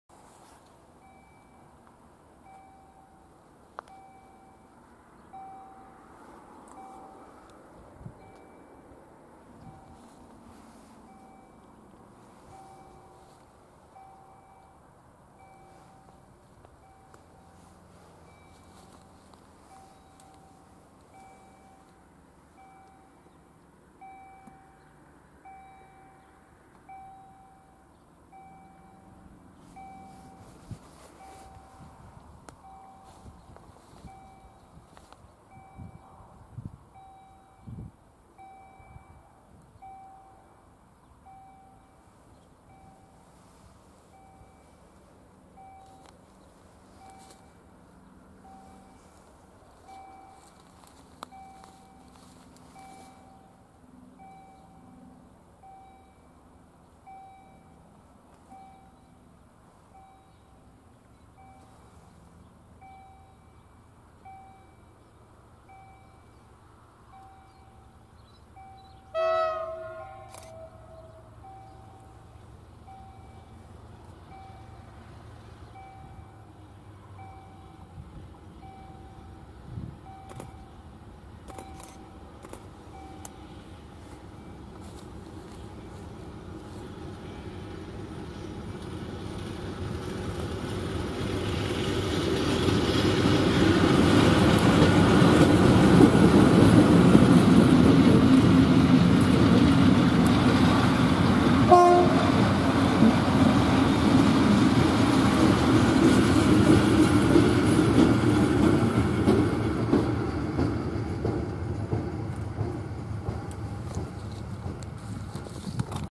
SM42-665+ET22-1023+ET41-142+SM42-722, Wola Klasztorna, 17.02.2019
A diesel shunting locomotive class SM42 hauling three locomotives.